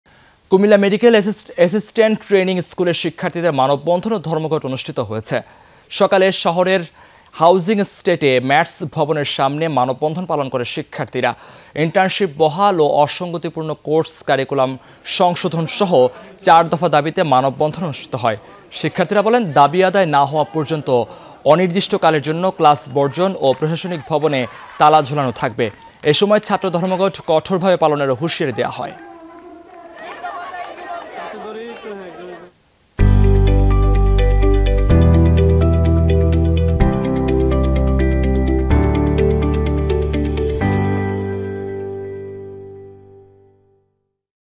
কুমিল্লা মেডিকেল এসিসট্যান্ট ট্রেনিং স্কুলের শিক্ষার্থীদের ধর্মঘট | Cumilla | MATS
কুমিল্লা মেডিকেল এসিসট্যান্ট ট্রেনিং স্কুলের শিক্ষার্থীদের মানববন্ধন ও ধর্মঘট অনুষ্ঠিত হয়েছে। সকালে শহরের হাউজিং এস্টেটে ম্যাটস ভবনের সামনে মানববন্ধন পালন করে শিক্ষার্থীরা।